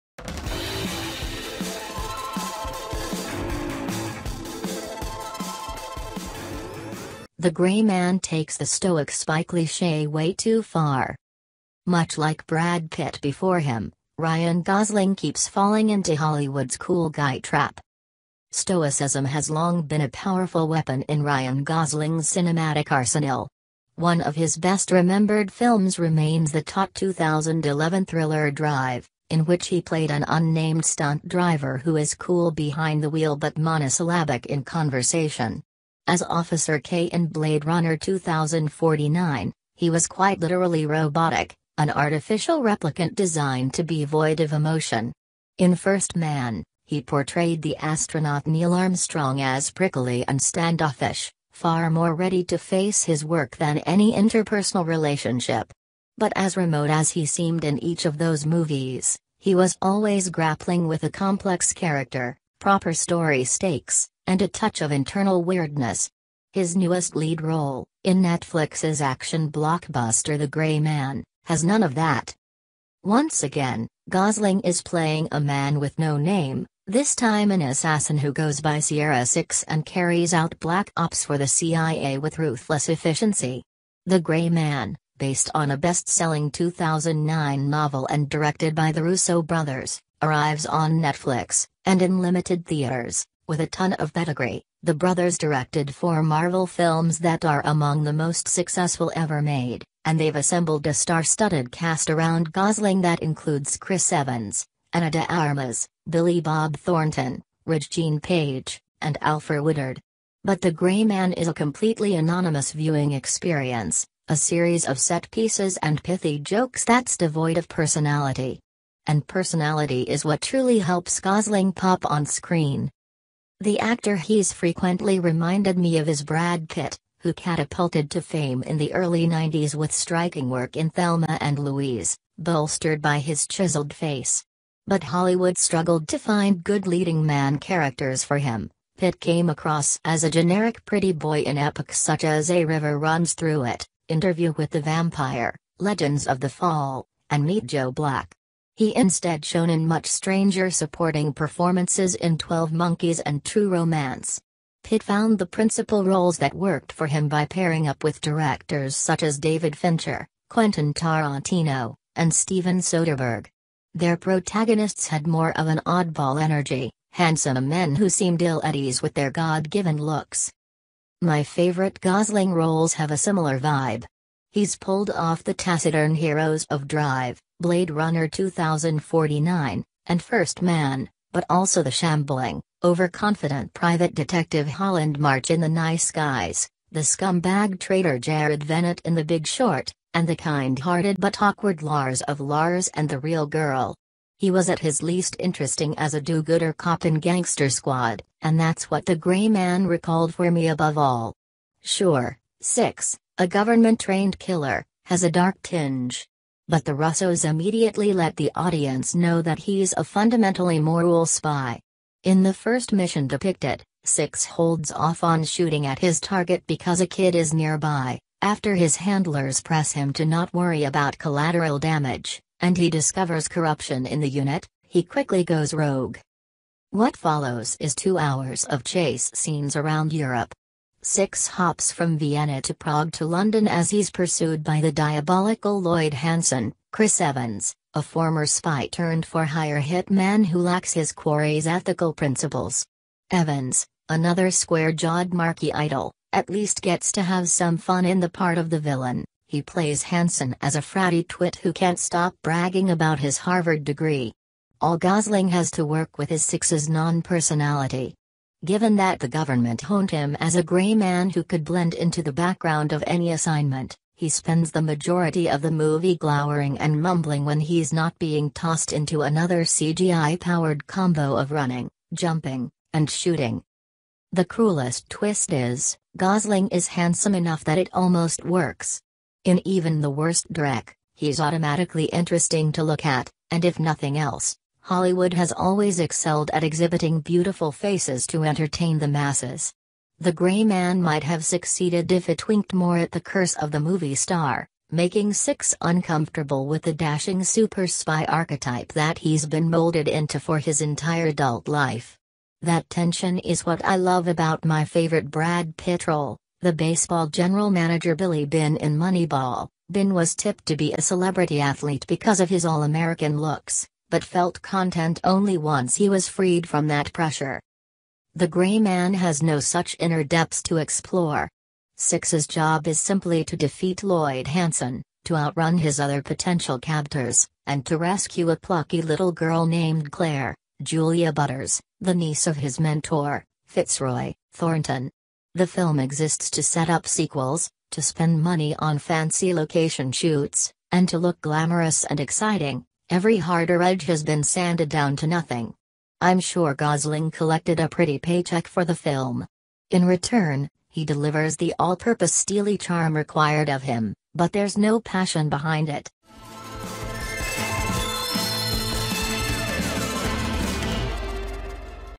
The Gray Man Takes the StoicSpy Cliché Way Too Far
Much like Brad Pitt before him, Ryan Gosling keeps falling into Hollywood's “cool guy” trap.
Stoicism has long been a powerful weapon in Ryan Gosling’s cinematic arsenal. One of his best-remembered films remains the taut 2011 thriller Drive, in which he played an unnamed stunt driver who is cool behind the wheel but monosyllabic in conversation. As Officer K in Blade Runner 2049, he was quite literally robotic, an artificial “replicant” designed to be void of emotion. In First Man, he portrayed the astronaut Neil Armstrong as prickly and standoffish, far more ready to face his work than any interpersonal relationship. But as remote as he seemed in each of those movies, he was always grappling with a complex character, proper story stakes, and a touch of internal weirdness. His newest lead role, in Netflix’s action blockbuster The Gray Man, has none of that.
Once again, Gosling is playing a man with no name, this time an assassin who goes by “Sierra Six” and carries out black ops for the CIA with ruthless efficiency. The Gray Man, based on a best-selling 2009 novel and directed by the Russo brothers, arrives on Netflix (and in limited theaters) with a ton of pedigree: The brothers directed four Marvel films that are among the most successful ever made, and they’ve assembled a star-studded cast around Gosling that includes Chris Evans, Ana de Armas, Billy Bob Thornton, Regé-Jean Page, and Alfre Woodard. But The Gray Man is a completely anonymous viewing experience, a series of set pieces and pithy jokes that’s devoid of personality. And personality is what truly helps Gosling pop on screen.
The actor he’s frequently reminded me of is Brad Pitt, who catapulted to fame in the early ’90s with striking work in Thelma and Louise, bolstered by his chiseled face. But Hollywood struggled to find good leading-man characters for him—Pitt came across as a generic pretty boy in epics such as A River Runs Through It, Interview With the Vampire, Legends of the Fall, and Meet Joe Black. He instead shone in much stranger supporting performances in 12 Monkeys and True Romance. Pitt found the principal roles that worked for him by pairing up with directors such as David Fincher, Quentin Tarantino, and Steven Soderbergh. Their protagonists had more of an oddball energy, handsome men who seemed ill at ease with their God-given looks.
My favorite Gosling roles have a similar vibe. He’s pulled off the taciturn heroes of Drive, Blade Runner 2049, and First Man, but also the shambling, overconfident private detective Holland March in The Nice Guys, the scumbag trader Jared Vennett in The Big Short, and the kind-hearted but awkward Lars of Lars and the Real Girl. He was at his least interesting as a do-gooder cop in Gangster Squad, and that’s what The Gray Man recalled for me above all. Sure, Six, a government-trained killer, has a dark tinge. But the Russos immediately let the audience know that he’s a fundamentally moral spy. In the first mission depicted, Six holds off on shooting at his target because a kid is nearby; after his handlers press him to not worry about collateral damage, and he discovers corruption in the unit, he quickly goes rogue.